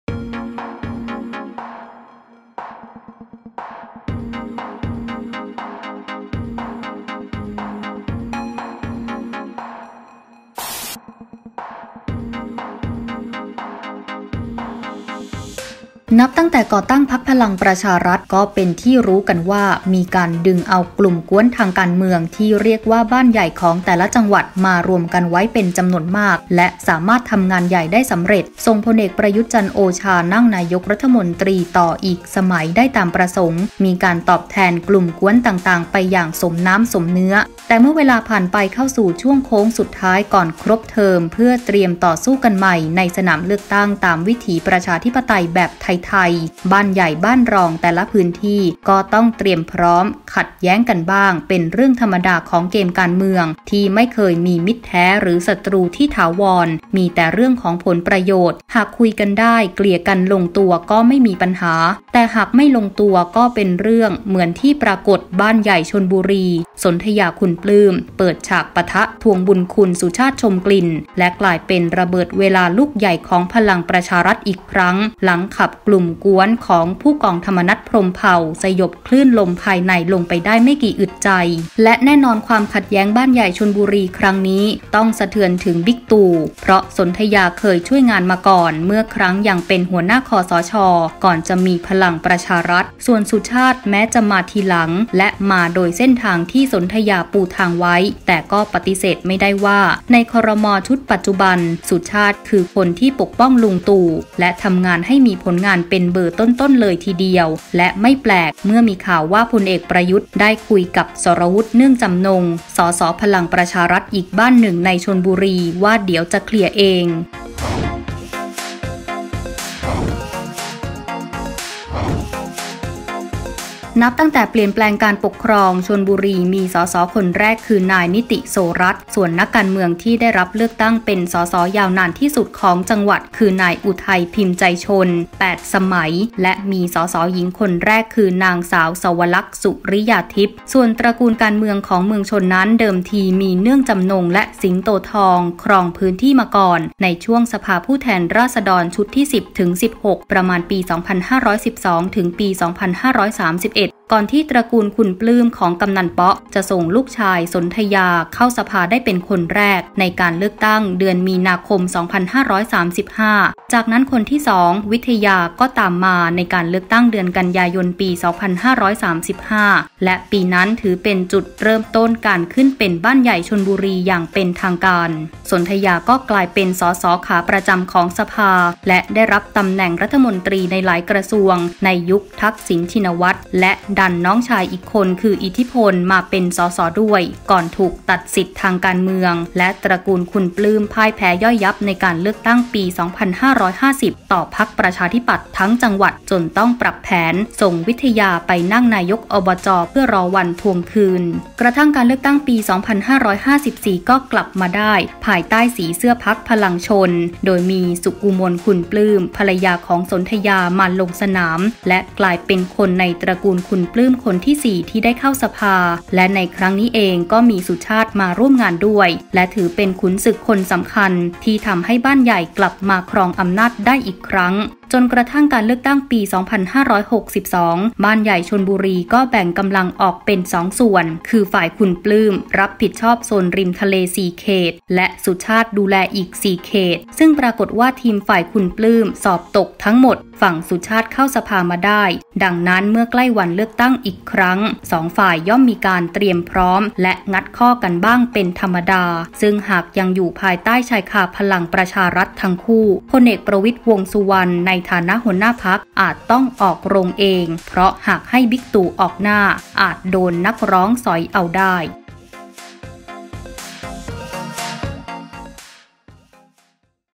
ศึกบ้านใหญ่ ชลบุรี ใครต้องเคลียร์
นับตั้งแต่ก่อตั้งพรรคพลังประชารัฐ ก็เป็นที่รู้กันว่า มีการดึงเอากลุ่มก๊วนทางการเมือง ที่เรียกว่าบ้านใหญ่ของแต่ละจังหวัด มารวมกันไว้เป็นจำนวนมาก และสามารถทำงานใหญ่ได้สำเร็จ ส่ง พล.อ.ประวิทธิ์ จันทร์โอชา นั่งนายกรัฐมนตรีต่ออีกสมัยได้ตามเป้าประสงค์ มีการตอบแทนกลุ่มก๊วนต่างๆ ไปอย่างสมน้ำสมเนื้อ